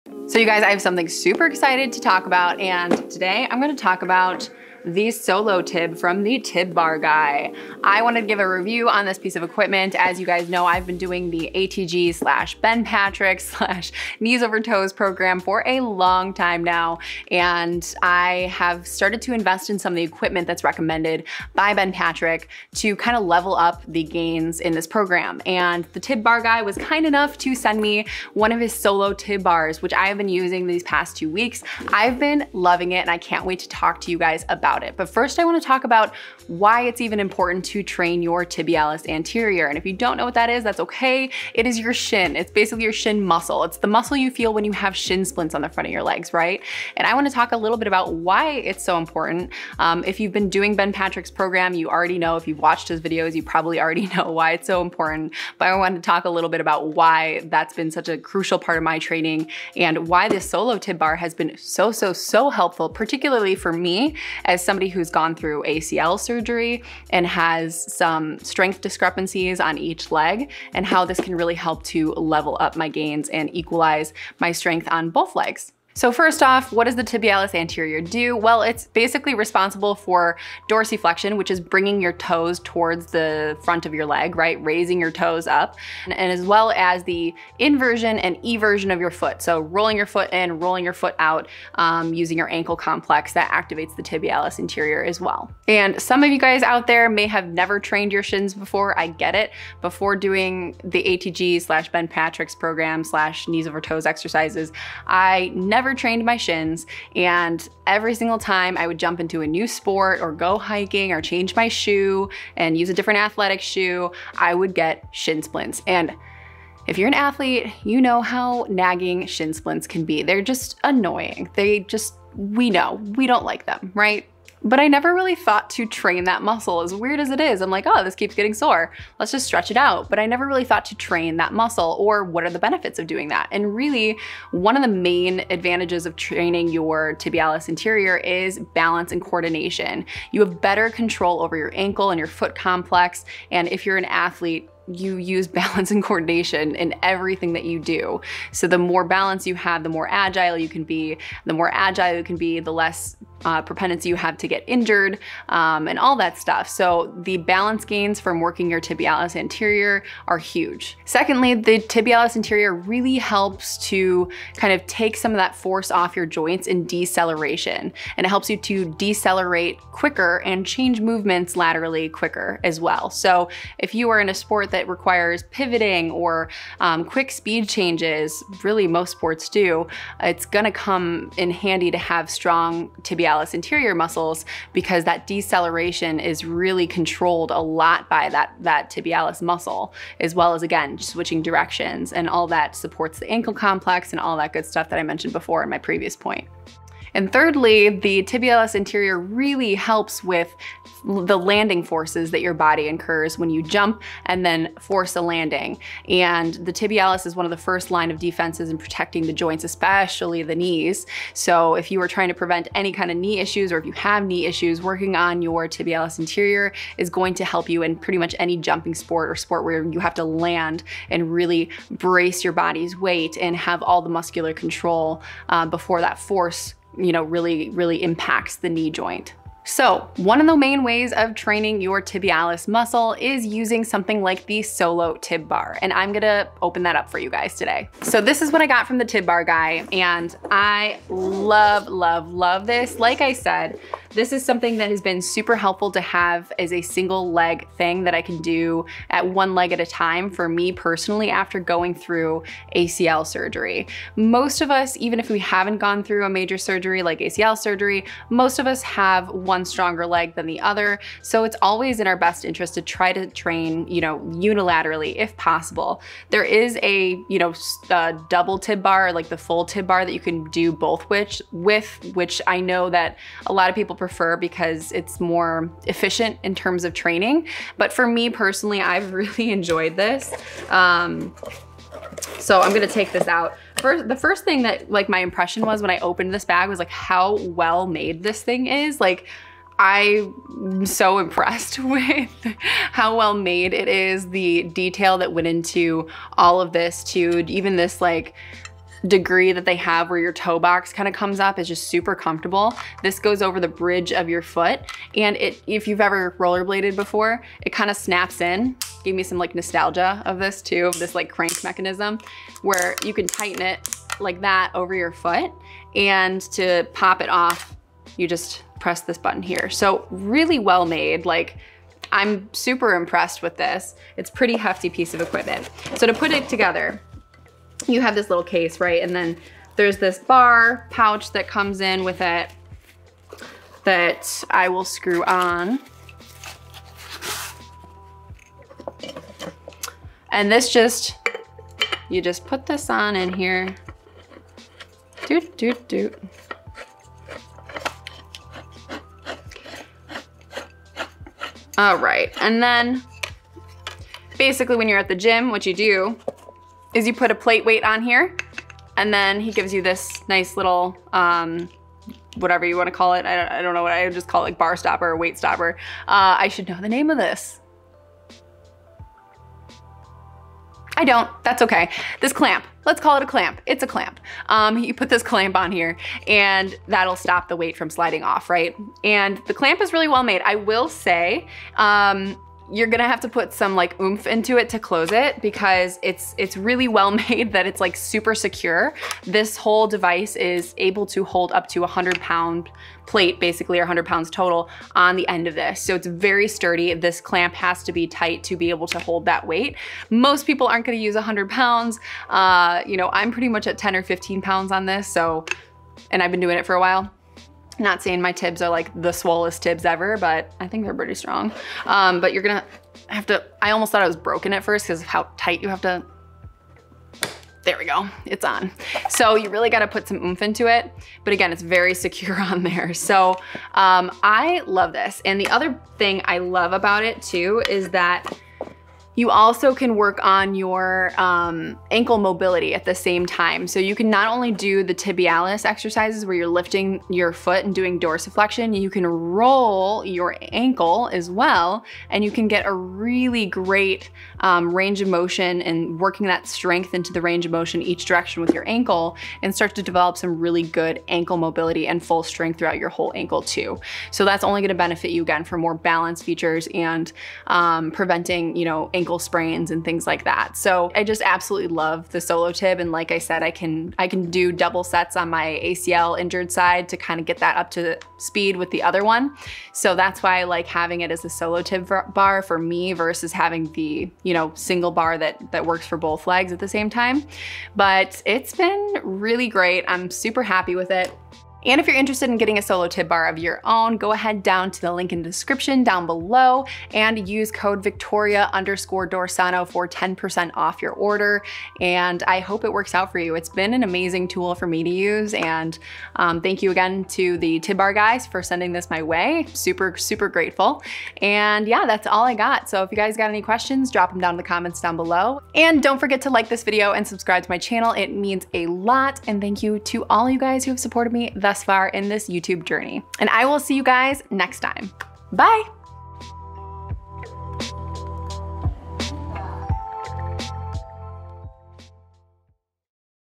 The BEST Equipment for Knee Pain - Solo Tib Bar Review
The Tibialis Anterior is such a crucial muscle in the leg that not many athletes pay attention to (until they get shin splints, that is).

And yes, if you've ever had a shin splint before, you know exactly where the Tibialis Anterior is. Right on the front of your shin.

But we often neglect this muscle, yet is is ABSOLUTELY CRUCIAL for helping our legs to decelerate, cut, and soften the load of landing from a jump. Without strong tibs, your knees end up taking a lot of the impact force, often causing a lot of knee pain. (And if you're an avid follower of @TheKneesovertoesguy , then you already know what I'm talking about here).

But for those of us who have had surgery on one knee already, there is often some strength discrepancy between the two legs. And that's where the Solo Tib Bar from @thetibbarguy is really a key player.

Check out the video to watch my full review of this amazing piece of equipment for knee pain resolution.

►TIME STAMPS:
0:00 Intro
1:17 The Role of the Tibialis Anterior
3:51 How to Train your Tibs with The Solo Tib Bar
7:41 Final Thoughts

[FREE Masterclass] 5 Steps to Conquer Your Goals